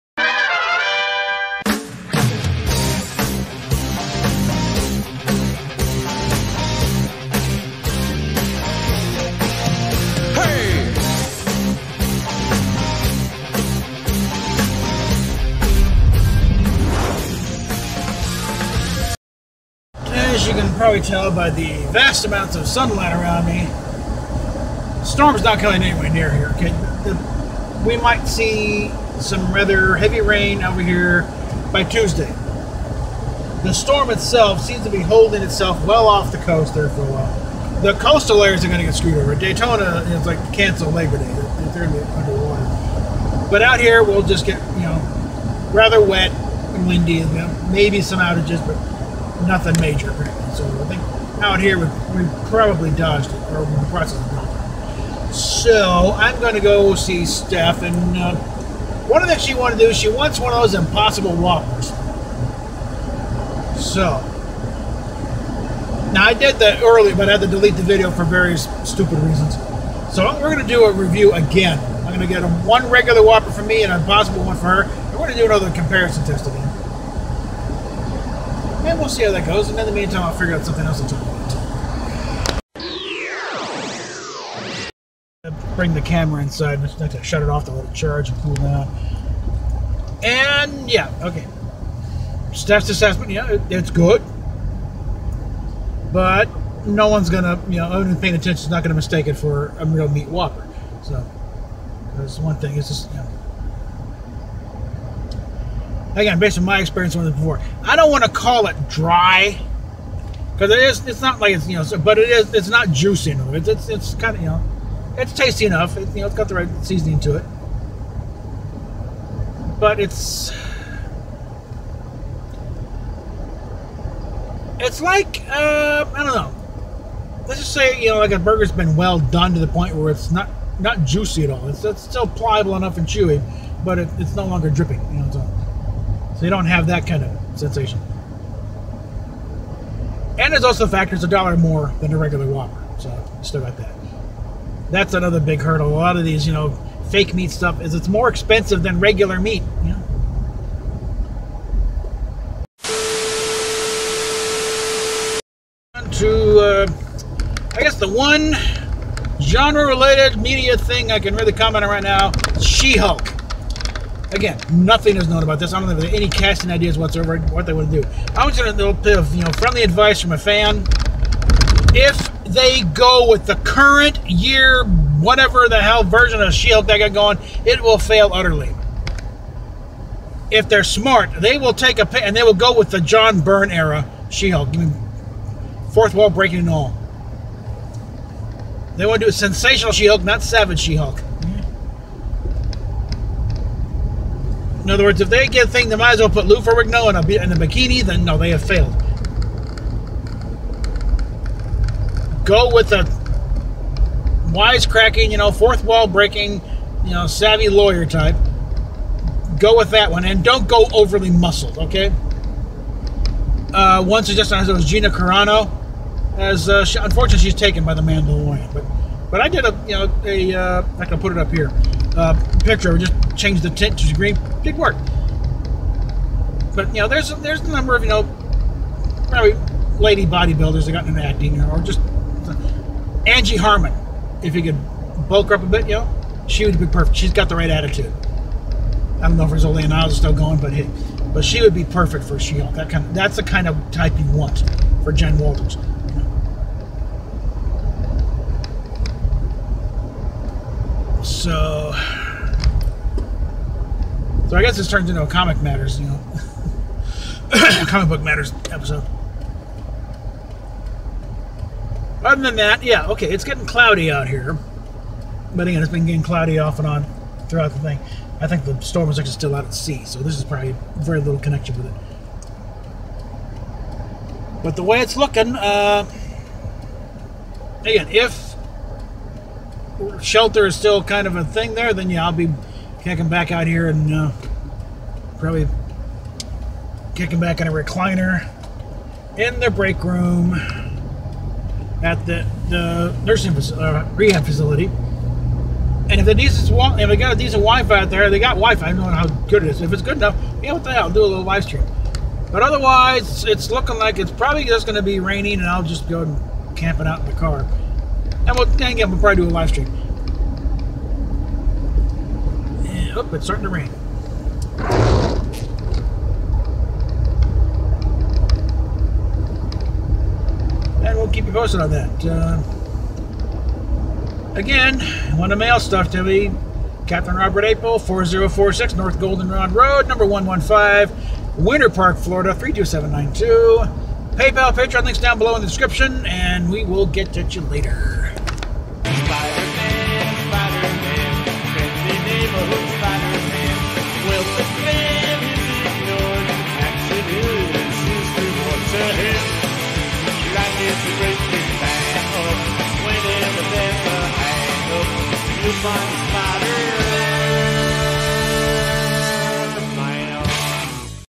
The Impossible Whopper...and some thoughts on She Hulk
Some thoughts on a big burger and big ol' piece of bright green cheesecake.

Wanna send me prezzies?

Capt Robert April
4046 N. Goldenrod Rd.

Petition to boot Vox Media from YouTube